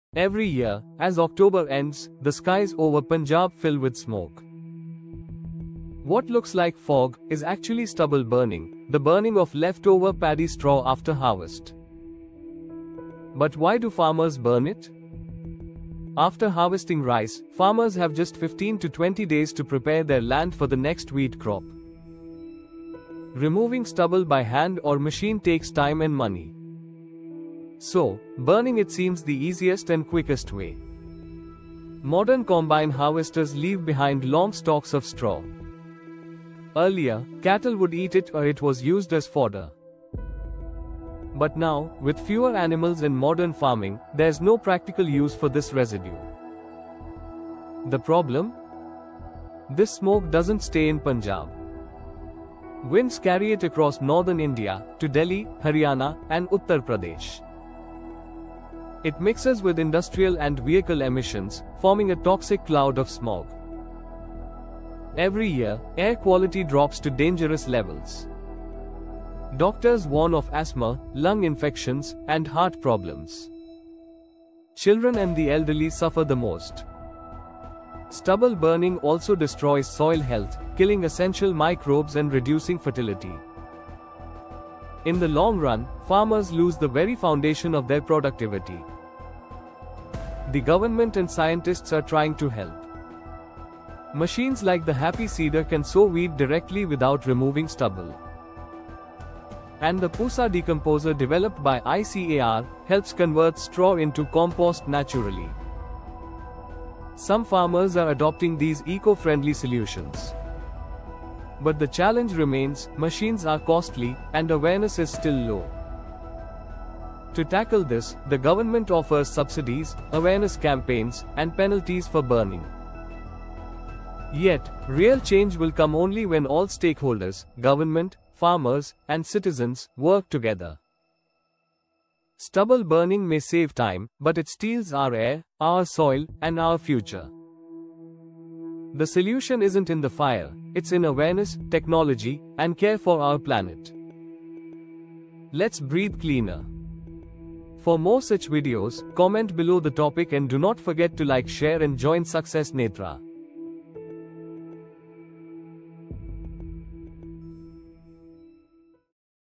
Stubble Burning in Punjab: The Smoke Behind India’s Pollution Crisis | Success Netra
Every year, Punjab’s fields turn smoky as farmers burn leftover paddy stubble — a quick fix that fuels India’s pollution crisis. 🌾🔥 In this video, we explain why stubble burning happens, its impact on air quality and health, and the solutions like the Happy Seeder and Pusa Decomposer that can help stop it.

Let’s understand the roots of the problem — and how awareness and innovation can clear the air for a cleaner, healthier India. 🇮🇳🌍